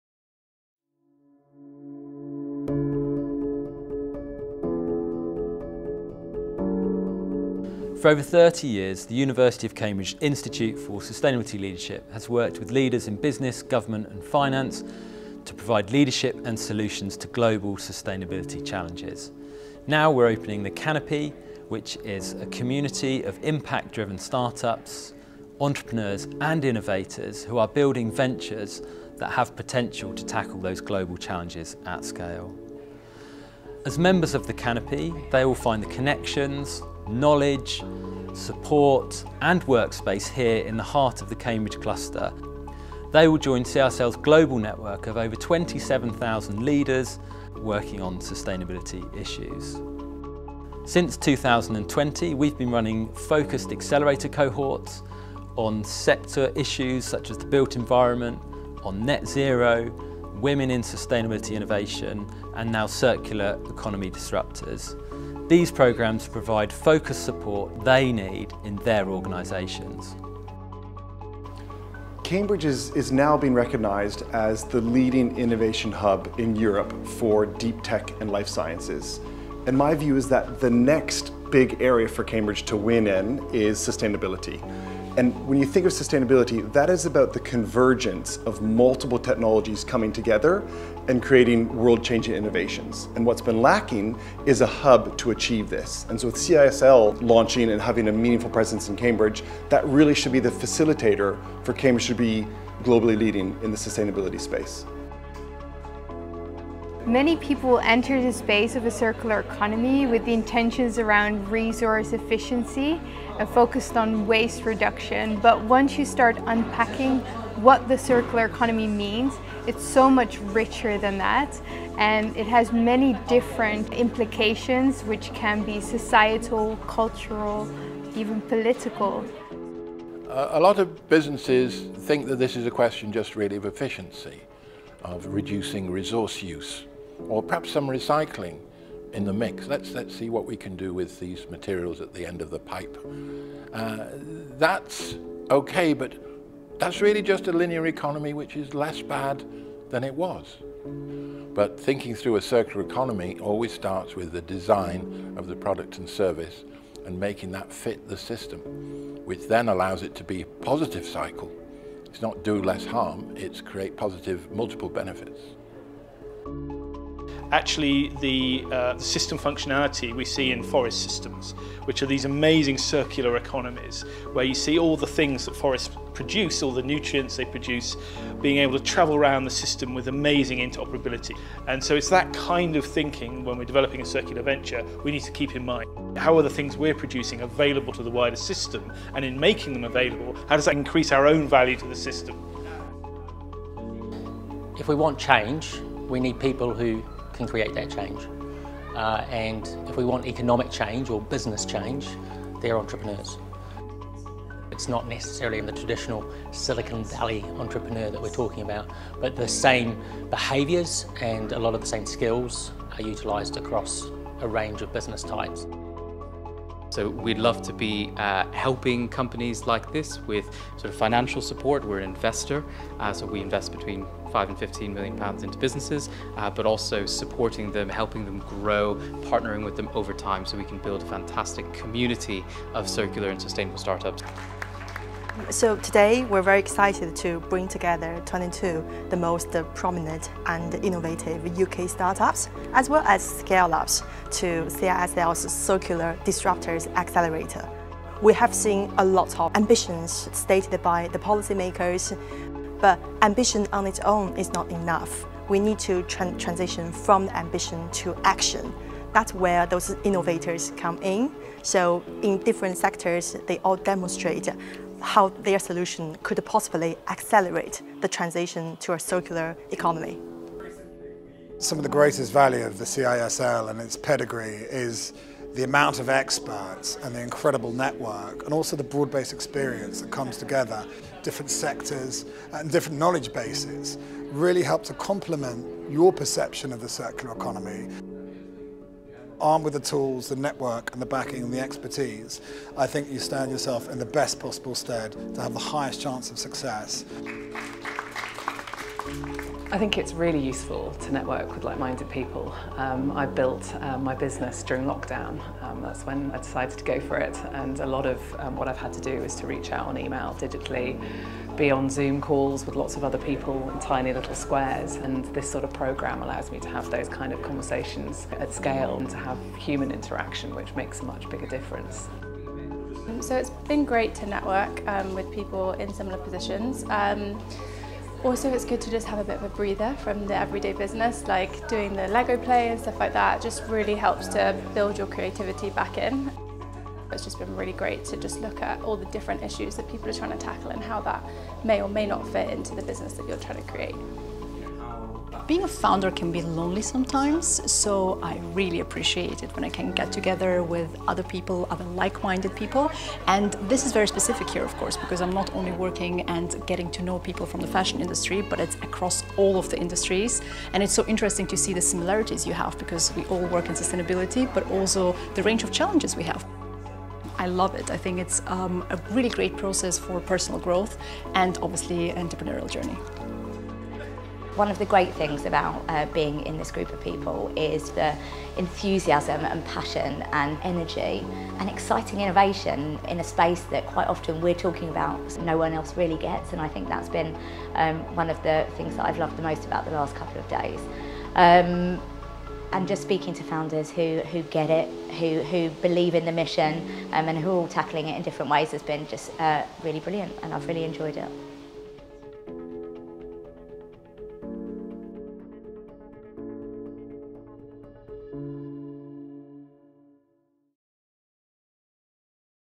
Perspectives from our Circular Disruptors Accelerator
Across the course of two days, our Circular Disruptors Accelerator brought together over twenty different founders who all have created their own sustainable innovations - from low-carbon alternatives to everyday items to disruptive solutions.

Our founders went through some intense training where radical thinking was encouraged, and they had the chance to perfect their product pitches.